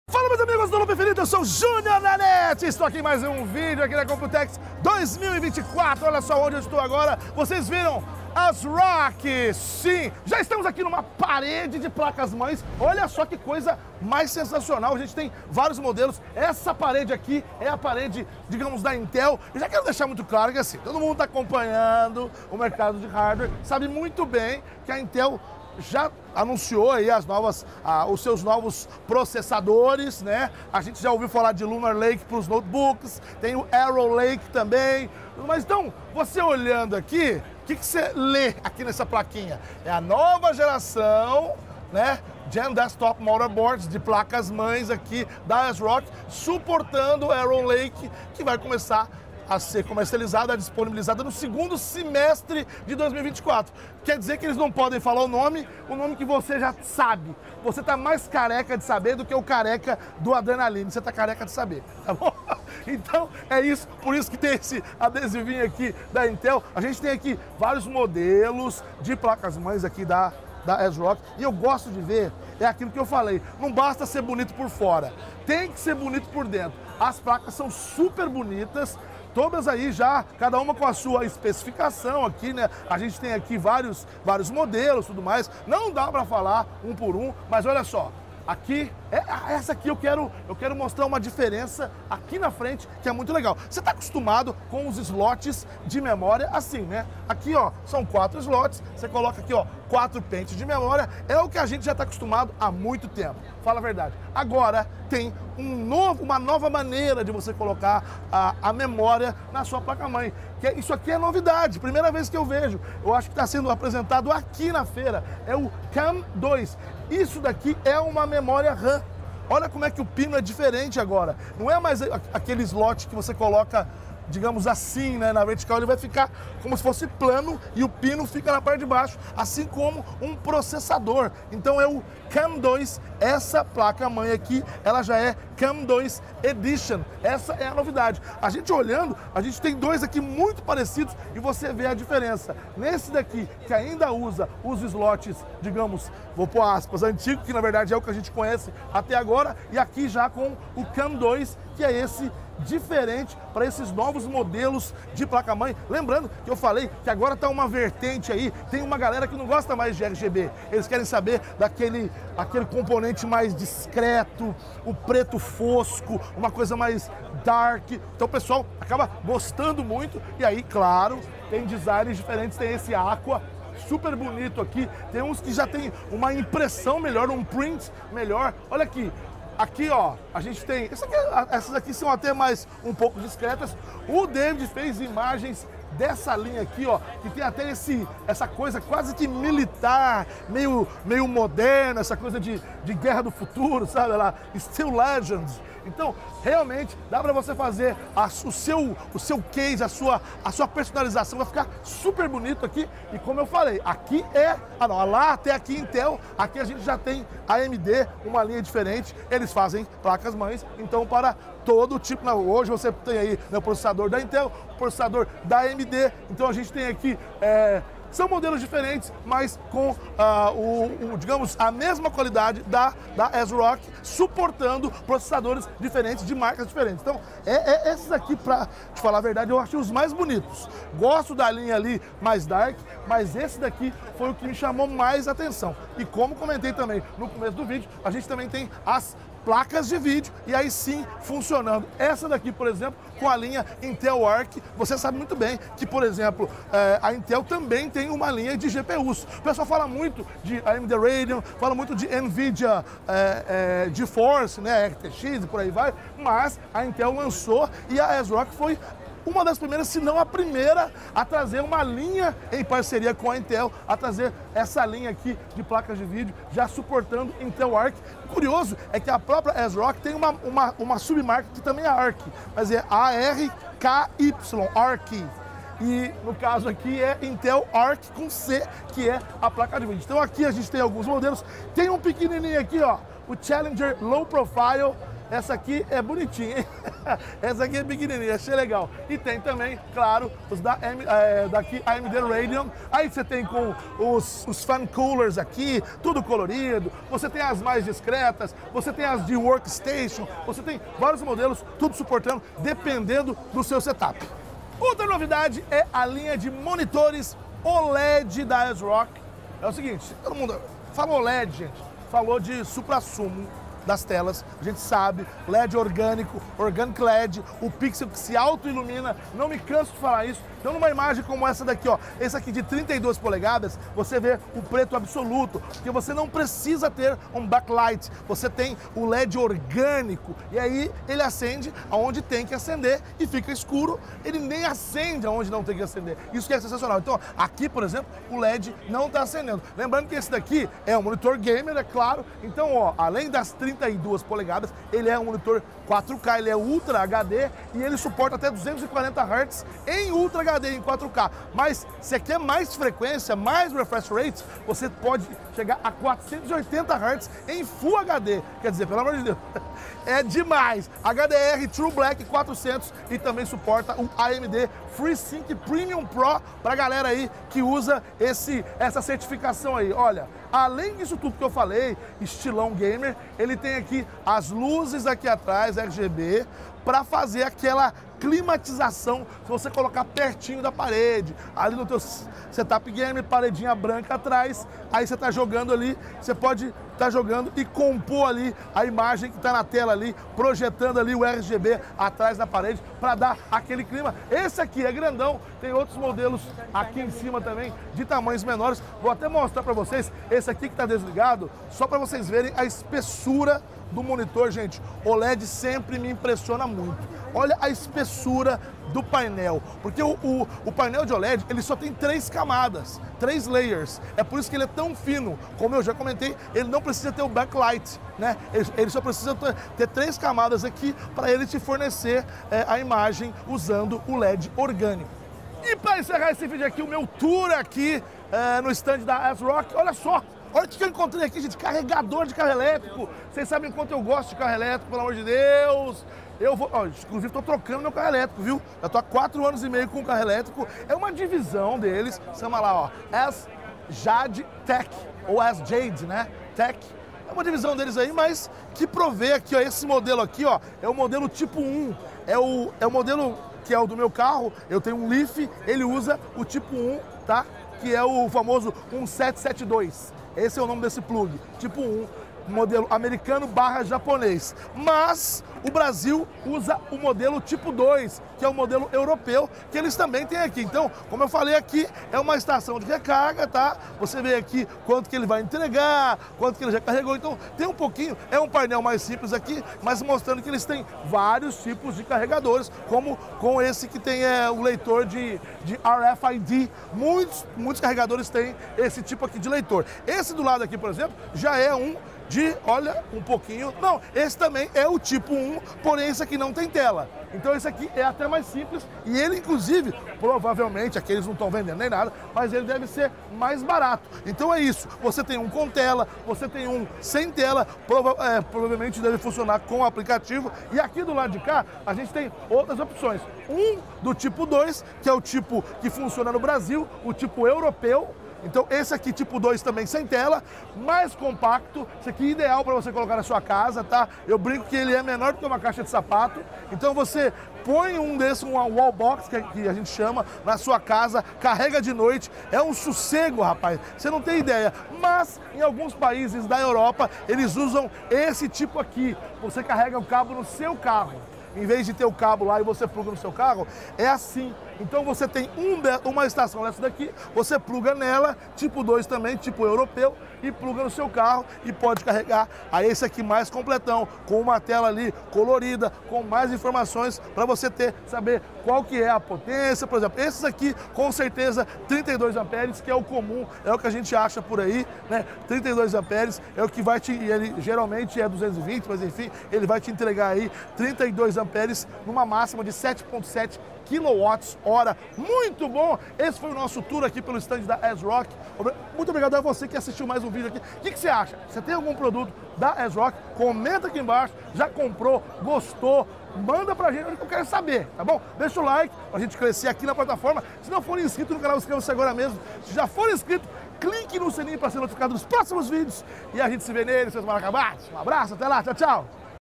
As NOVAS PLACAS da ASRock com memória CAMM2!
Na Computex 2024, a ASRock também apresentou novas placas com suporte para memórias CAMM2 e novos chipsets que ainda não foram anunciados pela Intel, além de monitores OLED e outras novidades que mostramos nesse vídeo!

00:00 - Introdução
00:10 - Placas Mãe
03:50 - Placas de vídeo
05:04 - Monitores OLED
07:04 - Carregadore de carro eletrico
9:34 - Encerramento